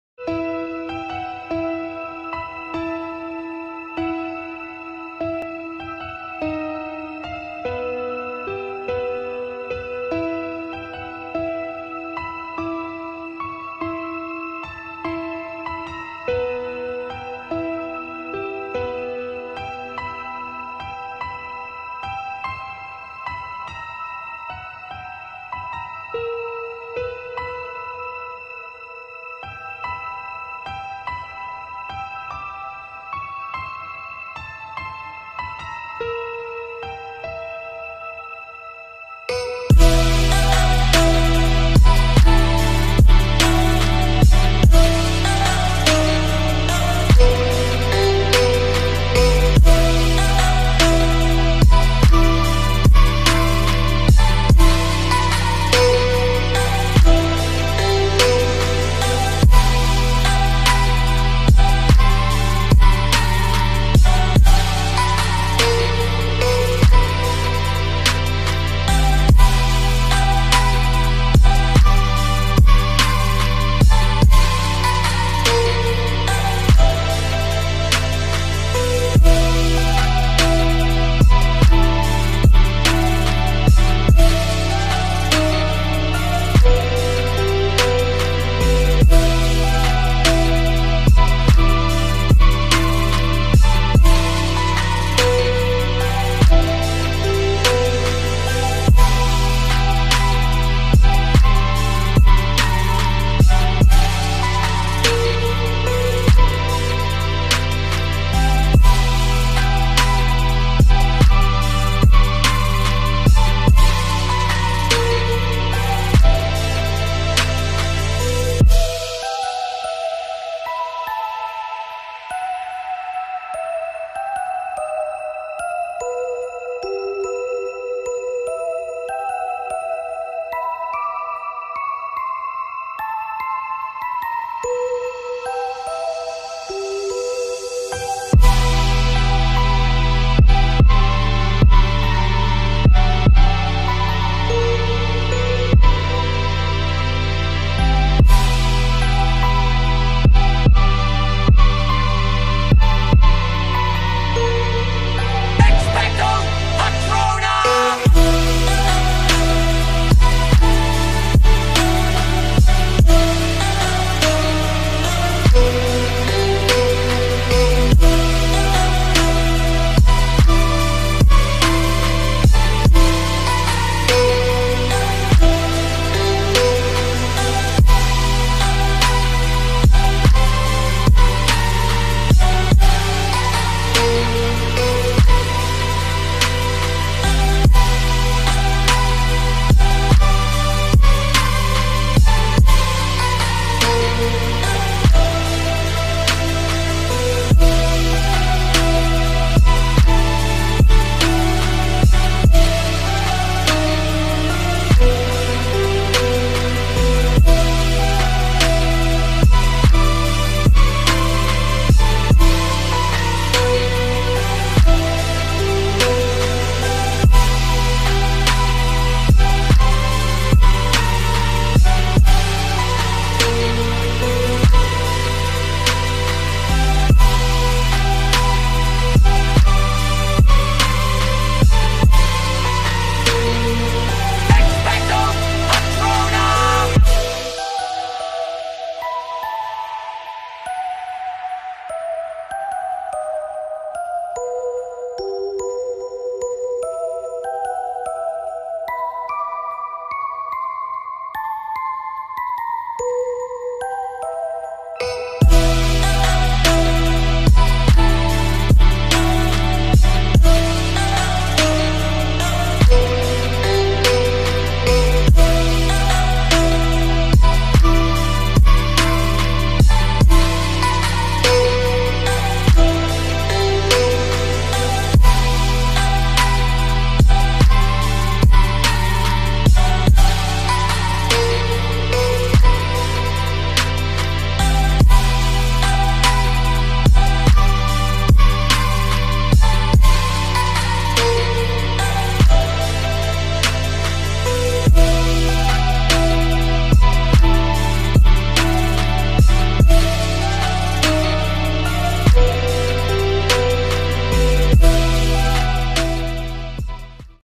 Making Hagrid's Hut on a random stock Image | Infinite Painter
Device used: Samsung Galaxy Tab S6 lite
Application: Infinite Painter
Image edited on Adobe Lightroom Mobile

I try modifying these stock images into illustrations. Let me know if you enjoyed seeing this transformation.